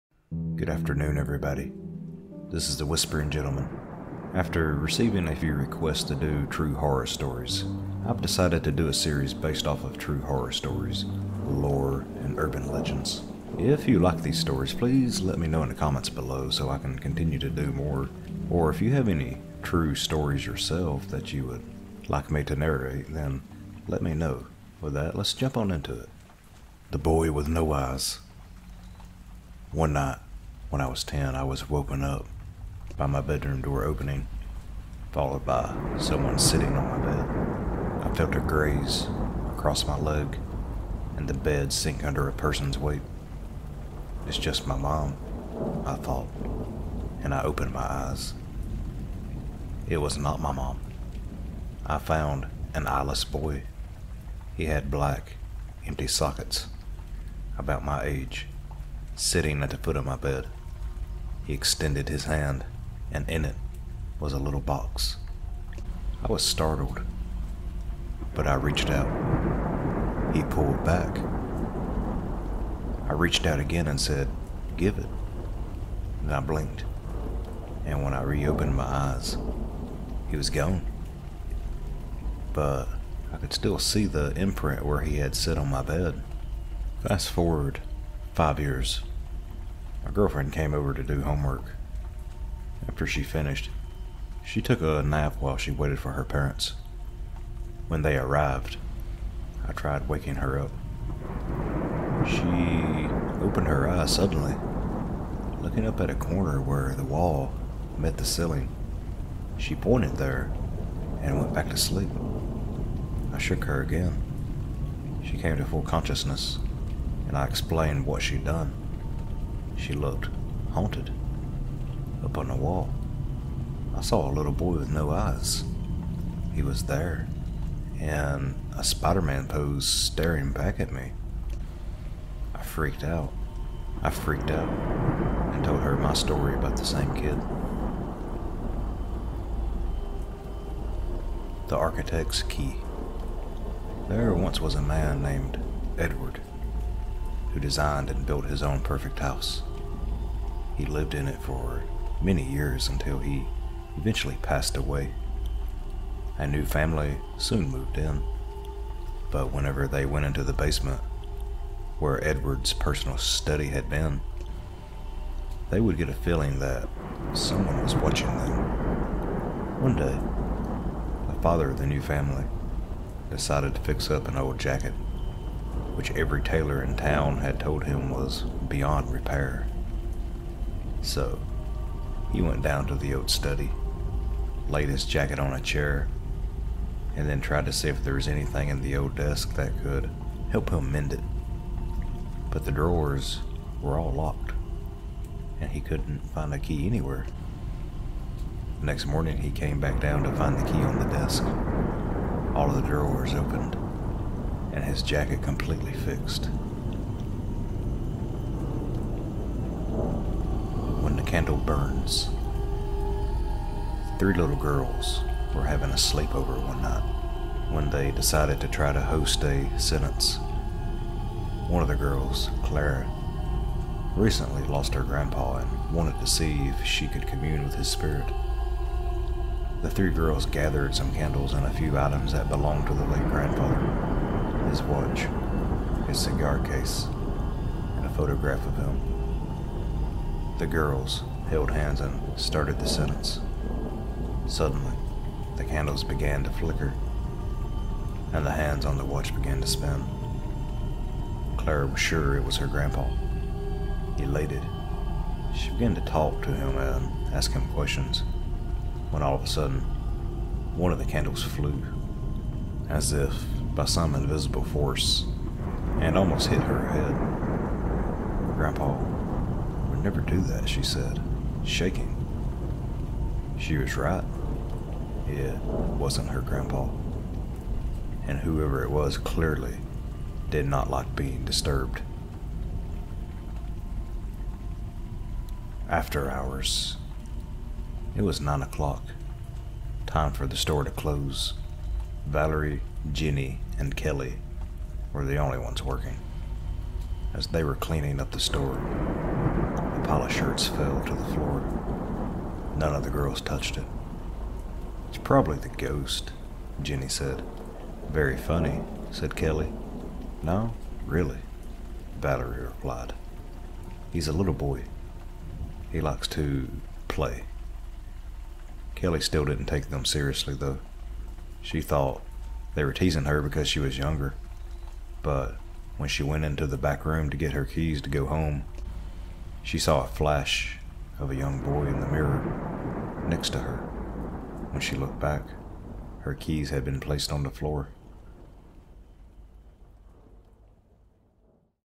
3 True Horror Stories
Stay Terrified or sleep tight tonight with these three ghostly horror stories